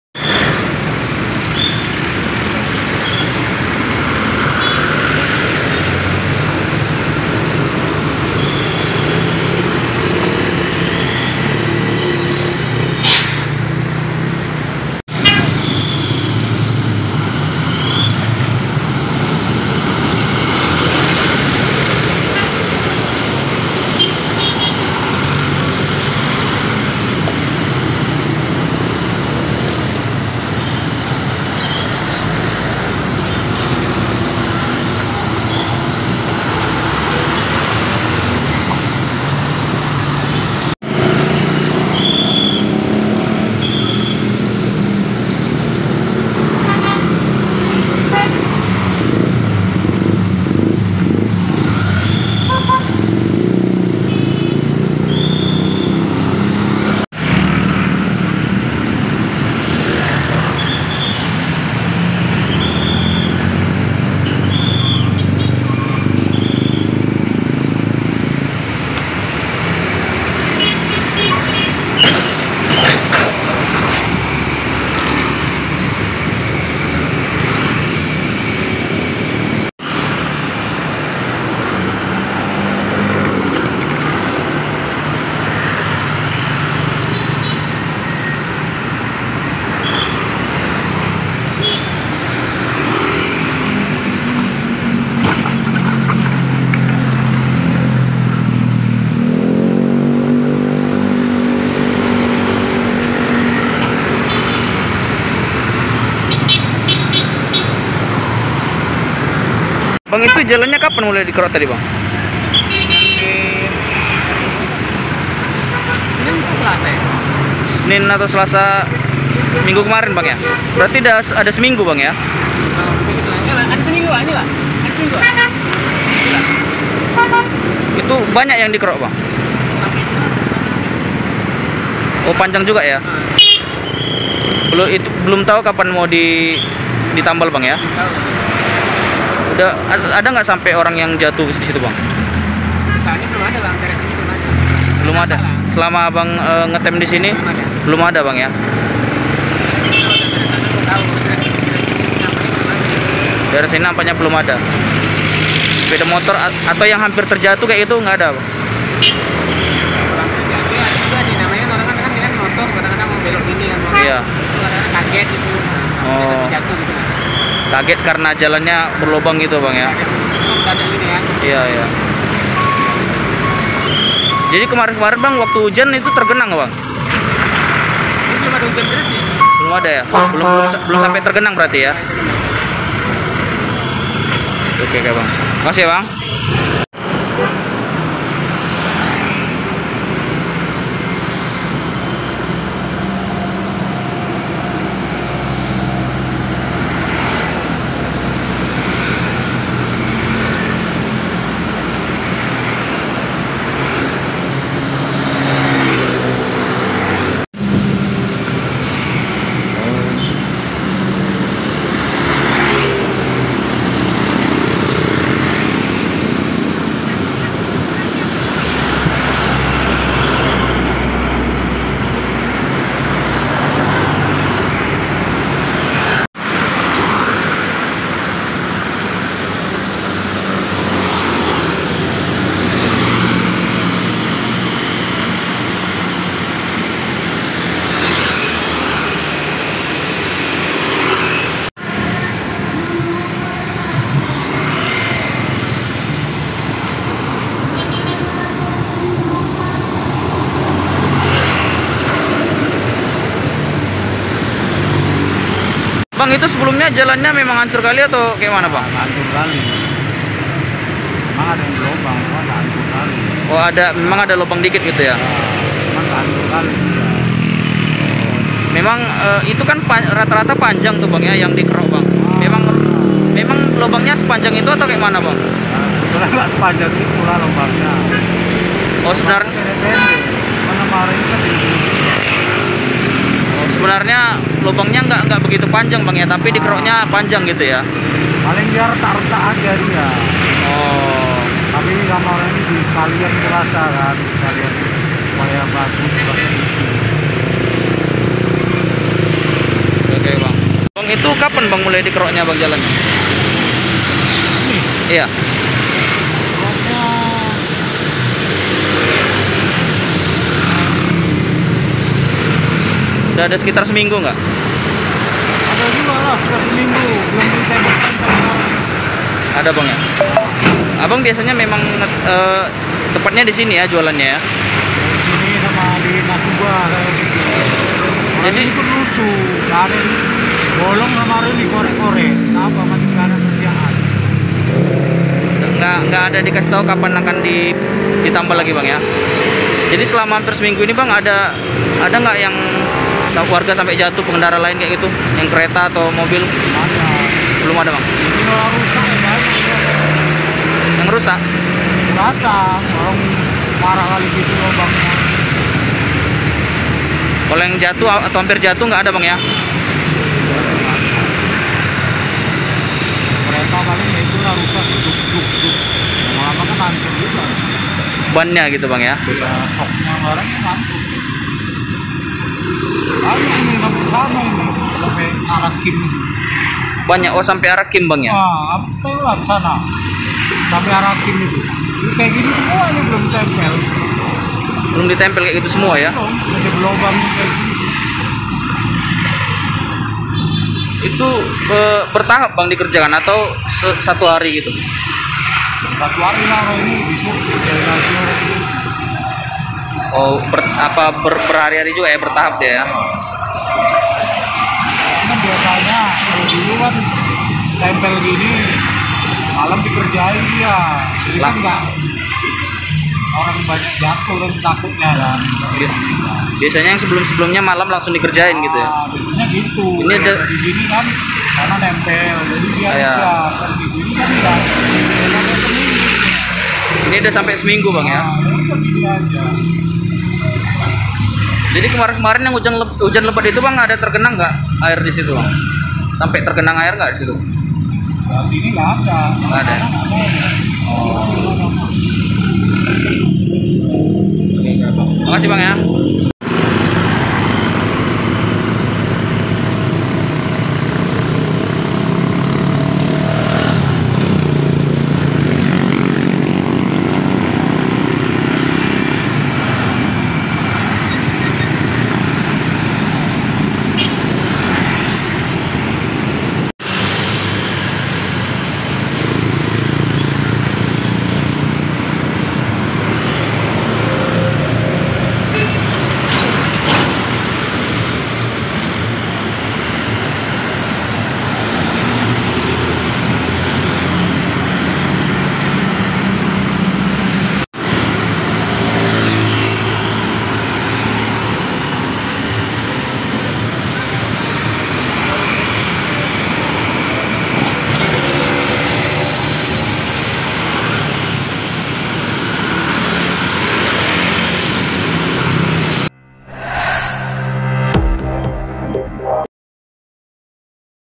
Sudah Seminggu Tambal Sulam di Jalan Kolonel Yos Sudarso Tak Kunjung Ditutup
TRIBUN-MEDAN.com, MEDAN - Sudah seminggu tambal sulam di Jalan Kolonel Yos Sudarso, Kelurahan Tanjung Mulia, Kecamatan Medan Deli tidak kunjung ditutup.

"Ini mulai dikeruk kayaknya hari senin minggu kemarin, sudah ada seminggu lah pokoknya," ujar Amin, Supir becak motor (Bentor), Selasa (15/6/2021).